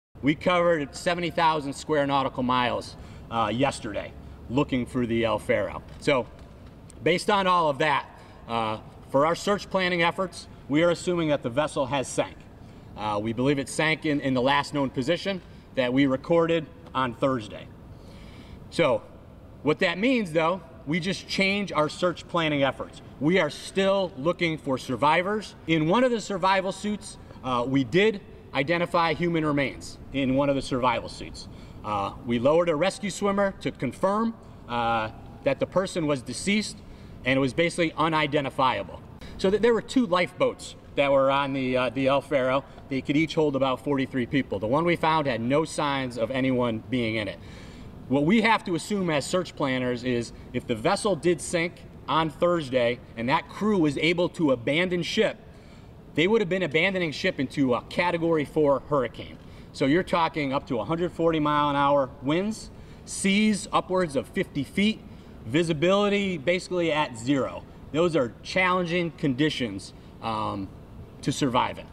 Coast Guard: Missing 'El Faro' Cargo Ship Sank
(5 Oct 2015) FOR CLEAN VERSION SEE STORY NUMBER: apus039645

The Coast Guard said Monday that a U.S. cargo ship carrying 33 people that has been missing since it encountered high winds and heavy seas from Hurricane Joaquin sank and one body was found, but planes and ships will continue searching for the missing crew.
Capt. Mark Fedor said one large debris field was spotted near the last known location of the 790-foot container ship El Faro near the Bahamas. The body, which Fedor said was "unidentifiable," was discovered in a survival suit, but no other human remains or survivors were immediately located.
Three Coast Guard cutters, two C-130 aircraft, helicopters, commercial tugboats and a U.S. Navy plane were searching across a wide expanse of Atlantic Ocean near Crooked Island in the Bahamas. Fedor said a heavily damaged lifeboat from the El Faro was discovered, but it had no people or signs of life. The ship had two lifeboats capable of holding 43 people each.
Fedor said it appears the vessel lost power, making it extremely vulnerable to the Category 4 hurricane's 140-mph winds and 50-foot waves. It had 391 shipping containers aboard, weight that could make the ship top-heavy and prone to capsizing, he added.
The El Faro departed from Jacksonville, Florida, on Sept. 29, when Joaquin was still a tropical storm. The ship had 28 crew members from the United States and five from Poland, and it was heading to Puerto Rico on a regular cargo supply run when it ran into trouble. Contact was lost early Thursday.
The crew reported that the ship had lost power, had taken on water and was listing 15 degrees but that the situation was "manageable," in their last communication Thursday morning, according to ship owner TOTE Maritime Puerto Rico.
They have not been heard from since.